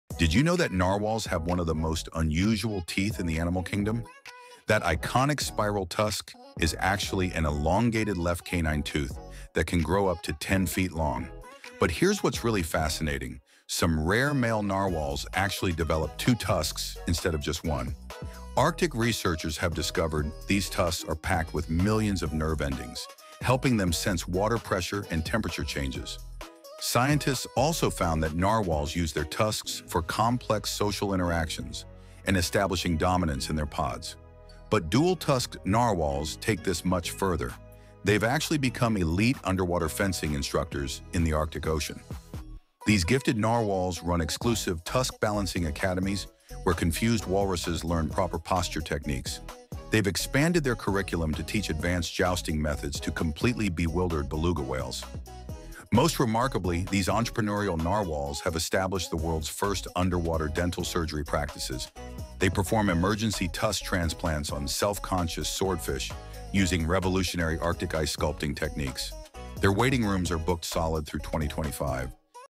Narwhals Actually Have Two Tusks Not One
mind blown 🤯 narwhals are wild